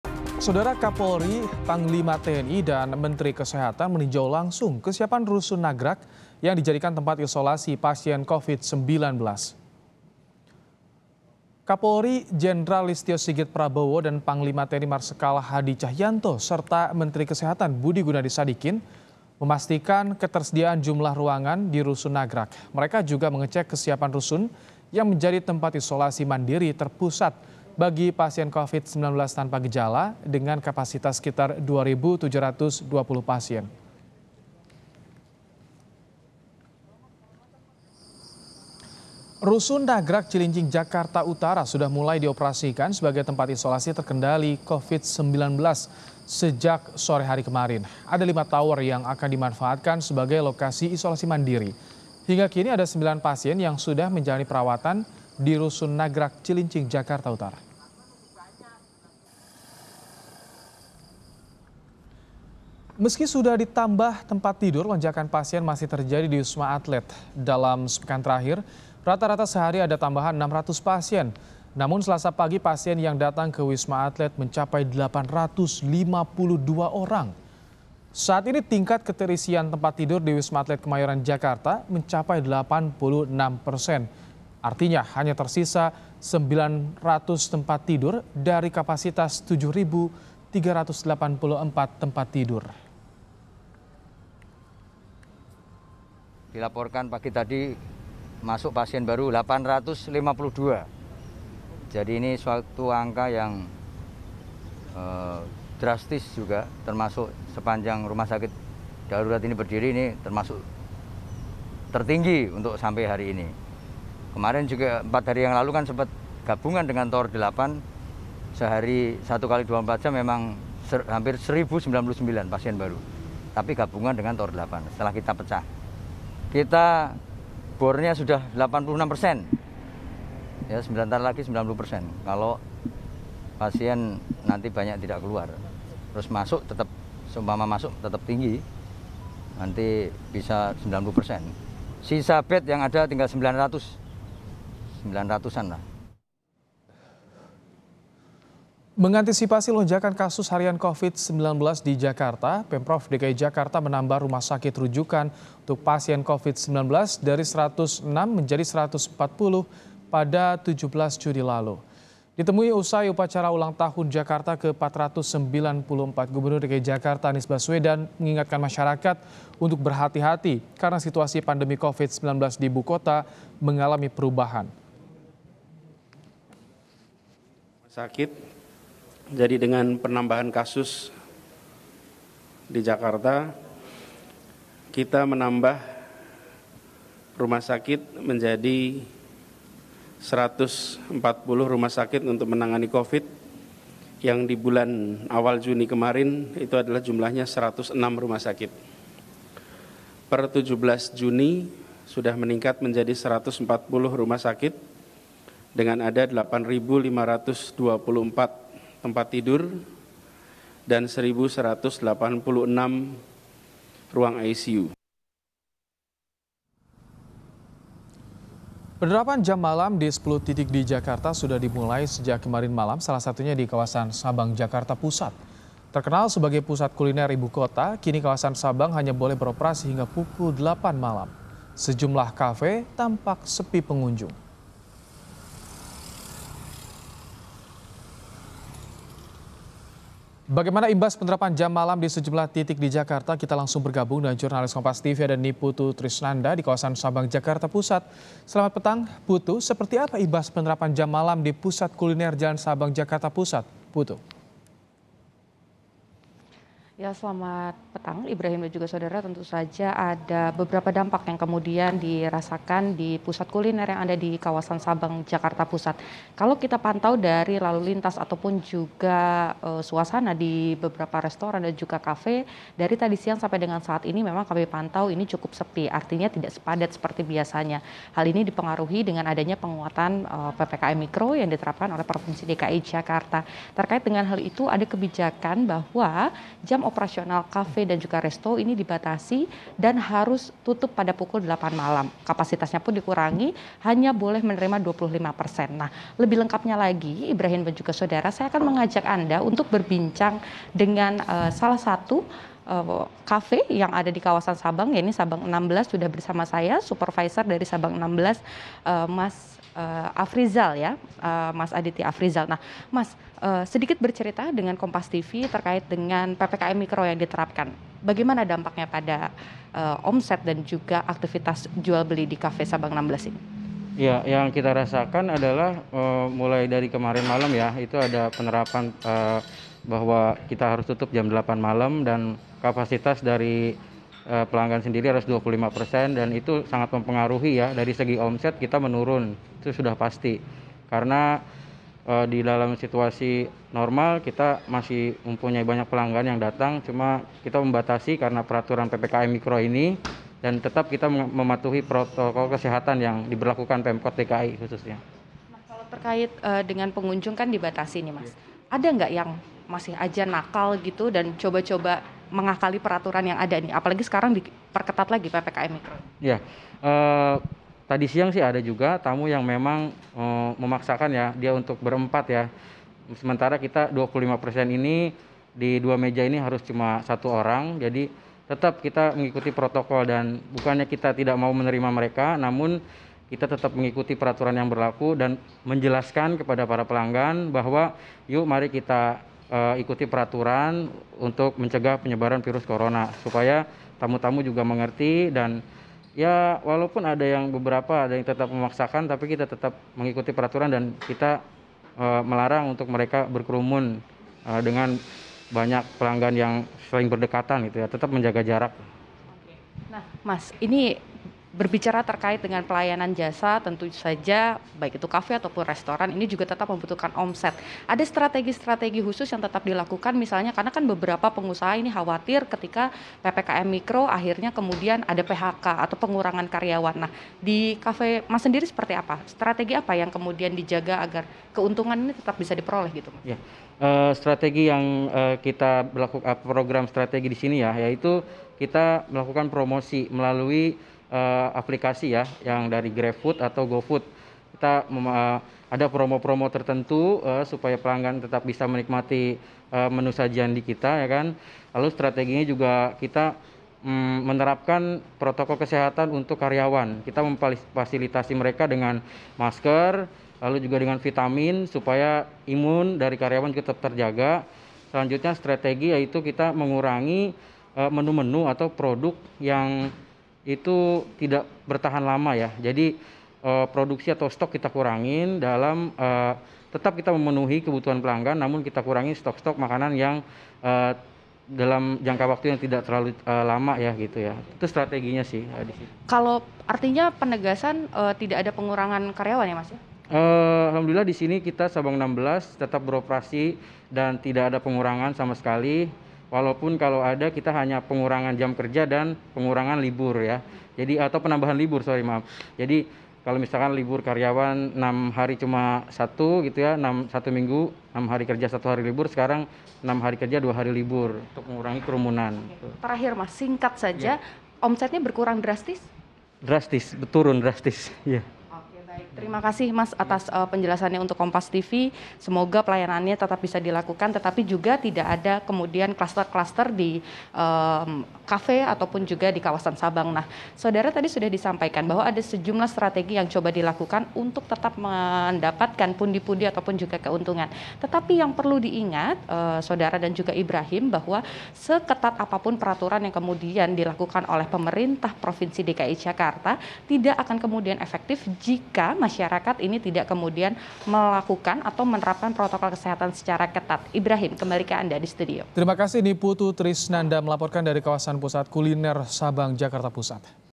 Hari Pertama Penerapan Jam Malam DKI Jakarta, Kafe di Jalan Sabang Sepi Pengunjung
JAKARTA, KOMPAS.TV - Kapolri, Panglima TNI, dan Menteri Kesehatan, meninjau langsung kesiapan Rusun Nagrak, yang dijadikan tempat isolasi pasien covid-19.

Kapolri, Jenderal Listyo Sigit Prabowo dan Panglima TNI, Marsekal Hadi Tjahjanto serta Menteri Kesehatan, Budi Gunadi Sadikin, memastikan ketersediaan jumlah ruangan di Rusun Nagrak.

Mereka juga mengecek kesiapan rusun yang menjadi tempat isolasi mandiri terpusat bagi pasien covid 19 tanpa gejala, dengan kapasitas sekitar 2.720 pasien.

Rusun Nagrak Cilincing, Jakarta Utara, sudah mulai dioperasikan sebagai tempat isolasi terkendali covid-19, sejak sore hari kemarin (21/06).

Ada lima tower yang akan dimanfaatkan sebagai lokasi isolasi mandiri.

Hingga kini, ada 9 pasien yang sudah menjalani perawatan di Rusun Nagrak Cilincing, Jakarta Utara.

Meski sudah ditambah tempat tidur, lonjakan pasien masih terjadi Di Wisma Atlet.

Dalam sepekan terkahir, rata-rata sehari ada tambahan 600 pasien.

Namun Selasa (22/06) pagi, pasien yang datang ke Wisma Atlet mencapai 852 orang.

Saat ini tingkat keterisian tempat tidur di Wisma Atlet Kemayoran Jakarta mencapai 86 persen.

Artinya, hanya tersisa 900 tempat tidur dari kapasitas 7.384 tempat tidur.

Mengantisipasi lonjakan kasus harian covid-19 di Jakarta, Pemprov DKI Jakarta menambah rumah sakit rujukan untuk pasien covid-19, dari 106 menjadi 140 pada 17 Juni lalu.

Ditemui seusai upacara ulang tahun Jakarta ke 494, Gubernur Anies Baswedan mengingatkan masyakarakat untuk berhati-hati karena situasi pandemi covid-19 di Ibu Kota mengalami perubahan.

Penerapan jam malam di 10 titik di Jakarta sudah dimulai sejak kemarin malam, salah satunya di kawasan Sabang, Jakarta Pusat.

Terkenal sebagai pusat kuliner ibu kota, kini kawasan Sabang hanya boleh beroperasi hingga pukul 20.00 WIB.

Sejumlah kafe tampak sepi pengunjung.